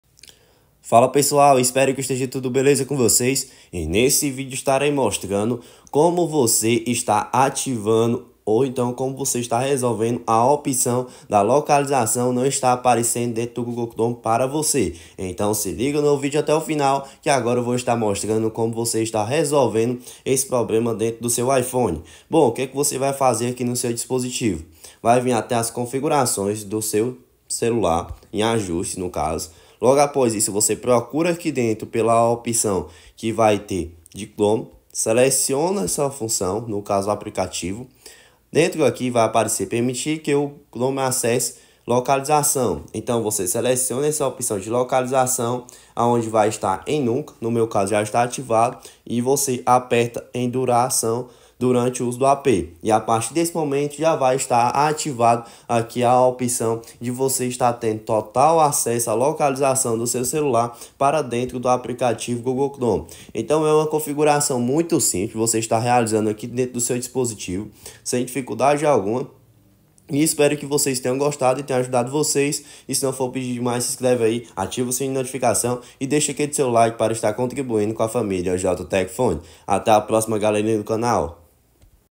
Como ligar a localização do iPhone no aplicativo google chrome
Eai galera.
Nesse vídeo você vai saber como está ligando a função da localização do celular iPhone no aplicativo google chrome.